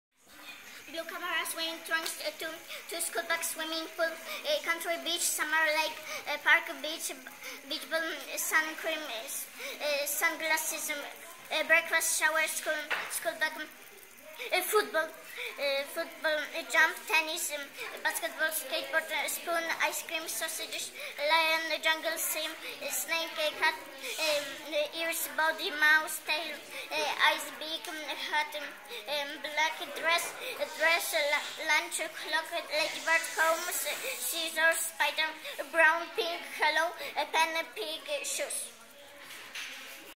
O tym jak Juleczka z 3d w pięknym stylu pokonała fidget spinner:-)
Język angielski w SP 118 we Wrocławiu